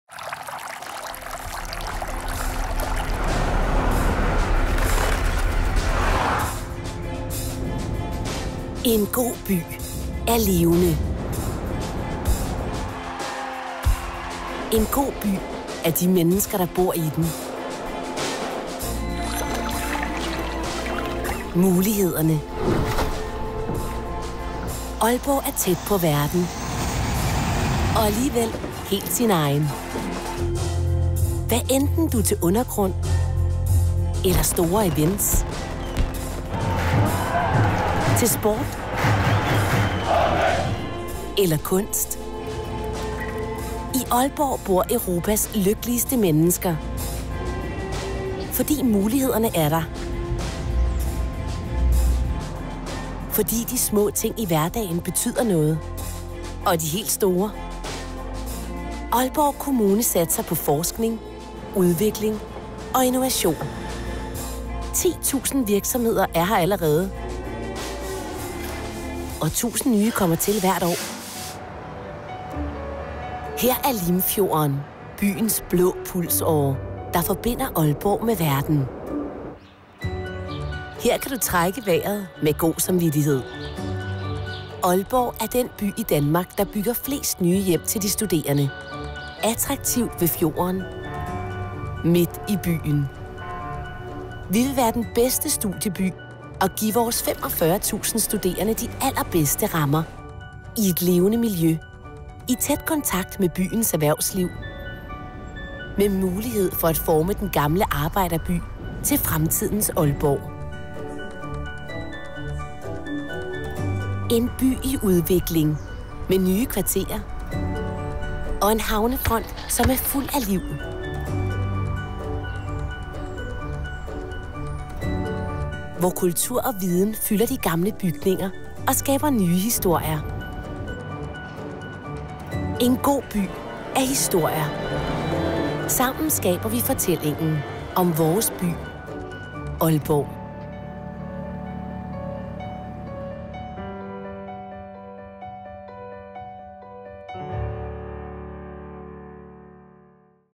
Aalborg Film
LANG VERSION:
Aalborg er en by i forvandling. Aalborg har gennem de seneste år fået rigtig mange nye ansigtstræk. Rekordmange studerende. Rekordmange nye ungdomsboliger og byggerier. Nye kulturinstitutioner. Nye iværksættere og nye arbejdspladser.